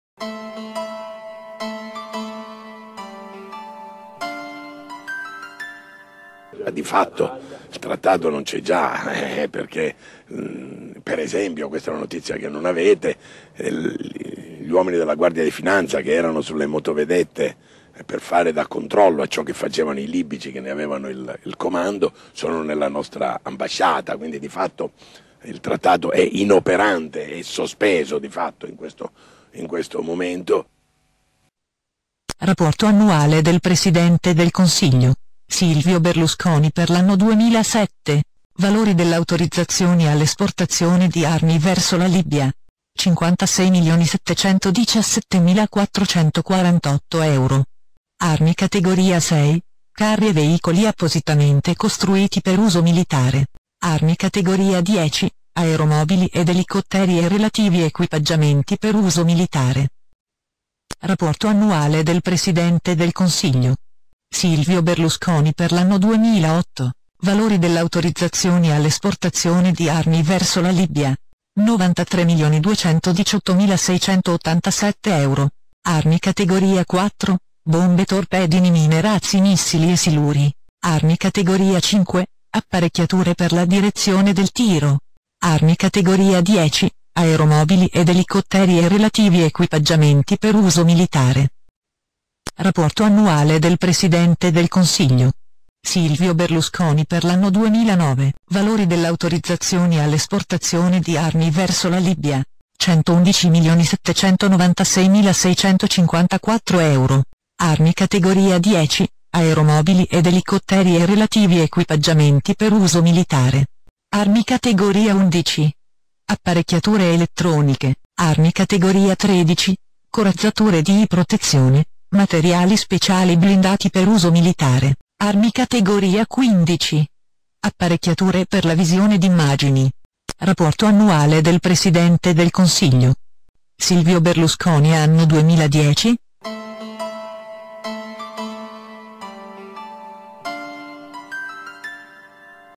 Libia - le armi italiane
- Rapporto annuale del Presidente del Consiglio Silvio Berlusconi,
valori delle autorizzazioni all'esportazione di armi verso la Libia.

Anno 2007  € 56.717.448
006 VEICOLI TERRESTRI carri e veicoli appositamente costruiti per uso militare
010 AEROMOBILI ( aeromobili ed elicotteri e relativi equipaggiamenti appositamente costruiti per uso militare)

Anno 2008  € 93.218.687
004 BOMBE, SILURI, RAZZI, MISSILI ED ACCESSORI (bombe,torpedini,mine,razzi,missili e siluri)
005 APPARECCHIATURE PER LA DIREZIONE DEL TIRO (sistemi o apparati elettronici, elettroottici e fotografici appositamente costruiti per uso militare)
010 AEROMOBILI ( aeromobili ed elicotteri e relativi equipaggiamenti appositamente costruiti per uso militare)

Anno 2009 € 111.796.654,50
004 BOMBE, SILURI, RAZZI, MISSILI ED ACCESSORI (bombe,torpedini,mine,razzi,missili e siluri)
010 AEROMOBILI ( aeromobili ed elicotteri e relativi equipaggiamenti appositamente costruiti per uso militare)

011 APPARECCHIATURE ELETTRONICHE (sistemi o apparati elettronici, elettroottici e fotografici appositamente costruiti per uso militare)

013 CORAZZATURE O EQUIPAGGIAMENTI DI PROTEZIONE E COSTRUZIONI (materiali speciali blindati appositamente costruiti per uso militare)
015 APPARECCHIATURE PER LA VISIONE D'IMMAGINI (sistemi o apparati elettronici, elettroottici e fotografici appositamente costruiti per uso militare).